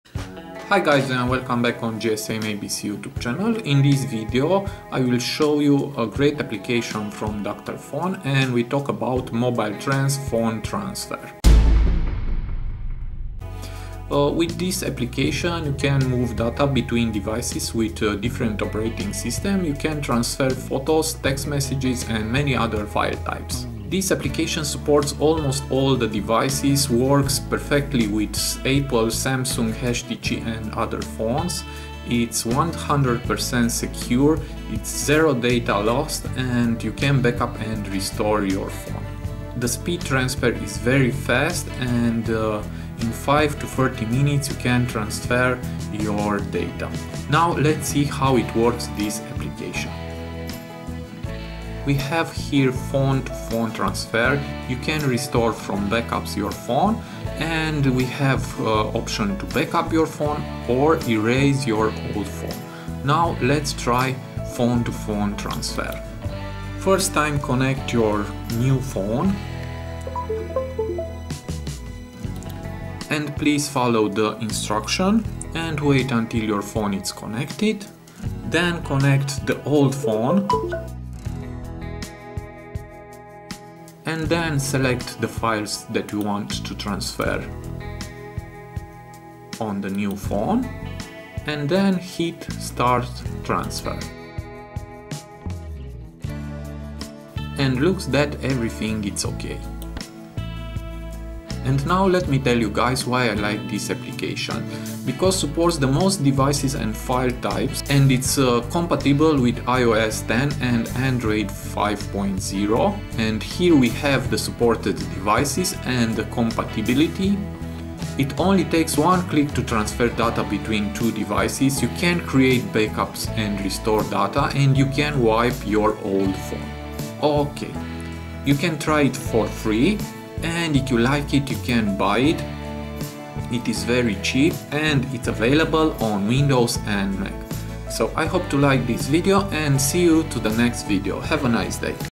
Best Way to Transfer Data from Android to a new Phone | MobileTrans Phone Transfer | Dr. Phone
In this video I will show you how to transfer your data between two phones with  MobileTrans application.